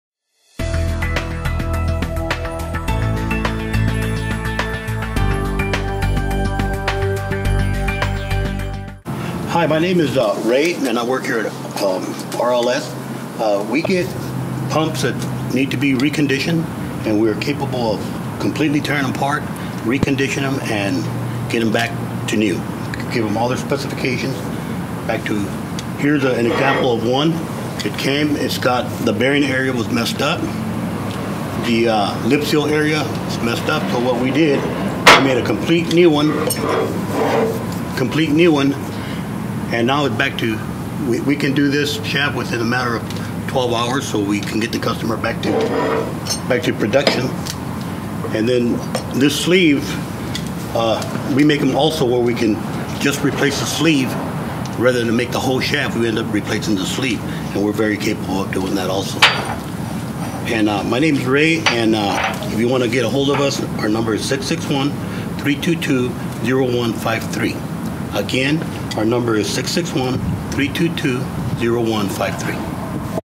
Ray on Refurbishing
Our machinist Ray talking about how our in house machine shop can help you get your equipment up and running again in short order, and how it's just one facet of how we here at RLS Industries have committed ourselves to being your one stop shop for pumps, compressors and valves.